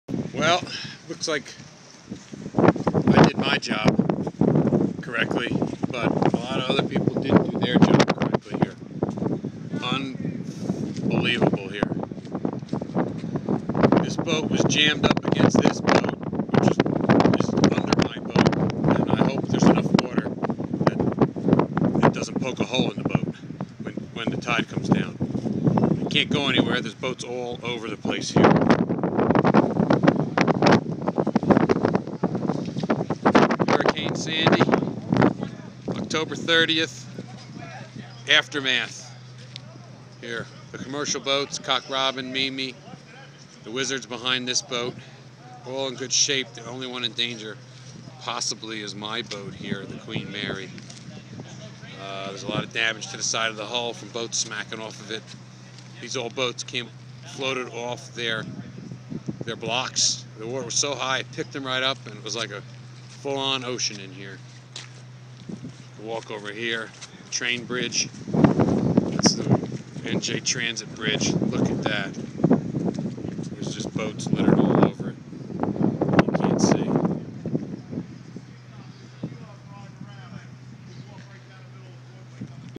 Aftermath Hurricane Sandy Queen Mary Dock Pt Pleasant Beach
Devastation, Destruction of the boats in Pt Pleasant Beach,NJ. Party Fishing Boat Queen Mary, Spike's Fishery.